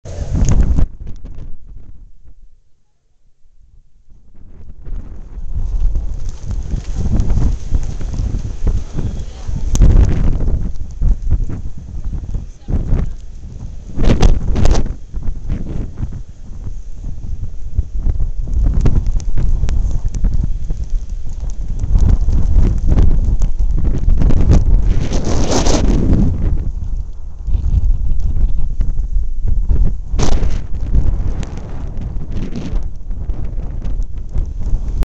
Dec 20 2008 - Bagdad Stowstorm
Yes, it is a snowstorm in Portland!